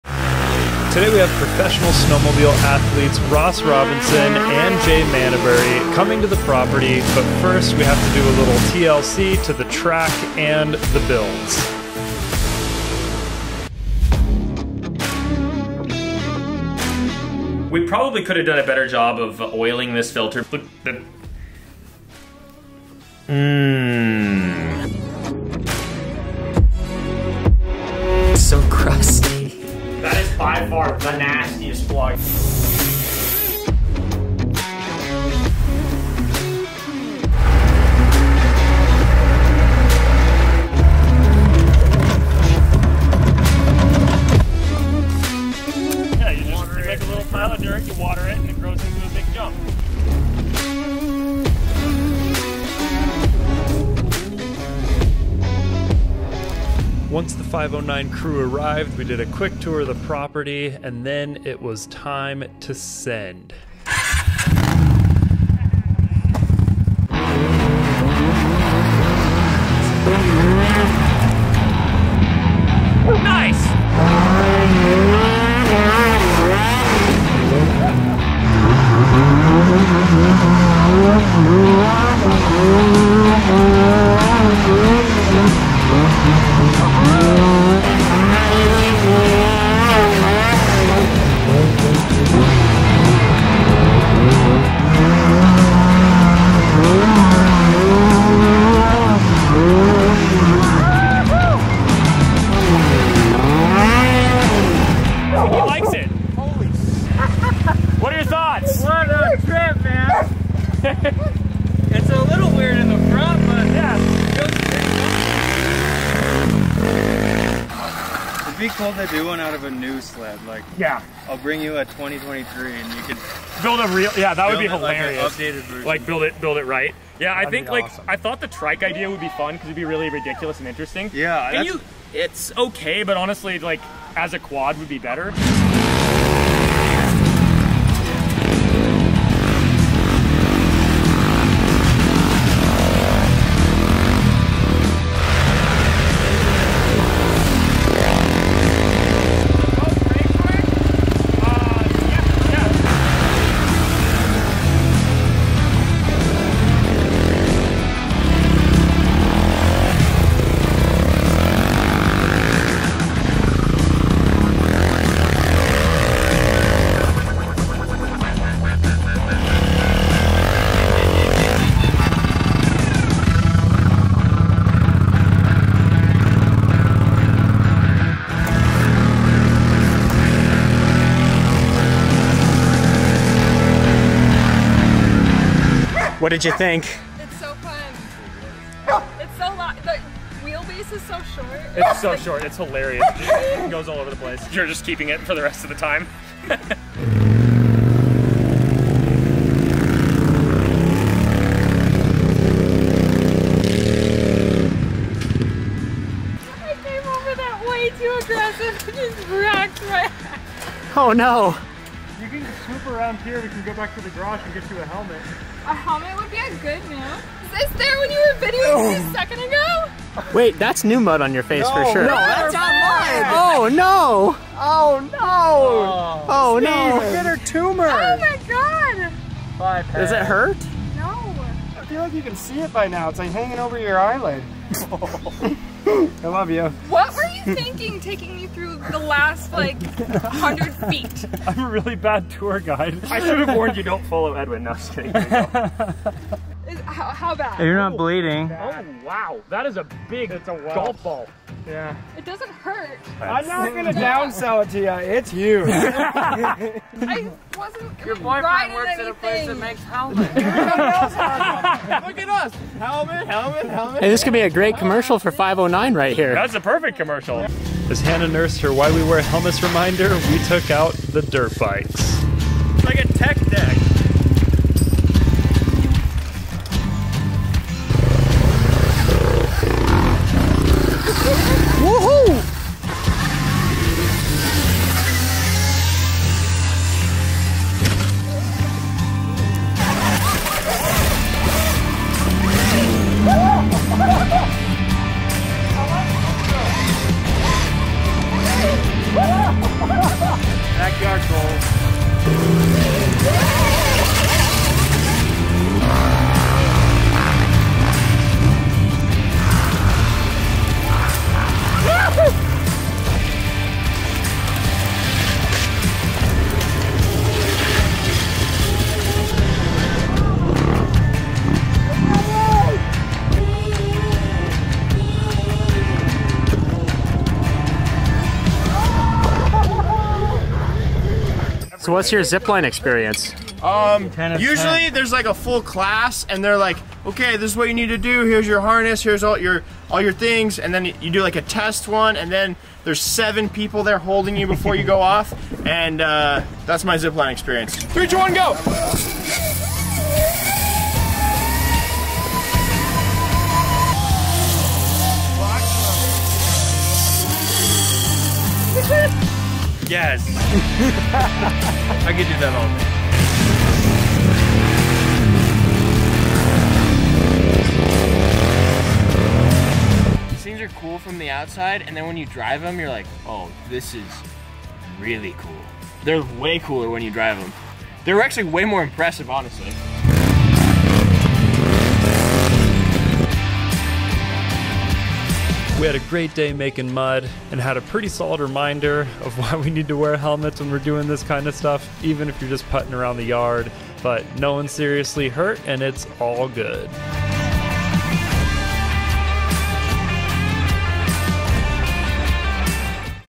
Insane Snowmobile Monstrosities Tear up Dirt! Tested by Pro Riders!
Professional snowmobile athletes test our dirt converted sleds! Out 1000cc Ski-Doo has a custom fabricated swing arm with a sport quad axle and ATV tracks. The Polaris Outlaw has a Timbersled kit and is an absolute blast! this was one great day with Professional snowmobile athletes Ross Robinson @ross.xr and Jay Mentaberry @jaymentaberry411. Special thanks to 509 for making this all happen!

Second Channel  @Sent and Bent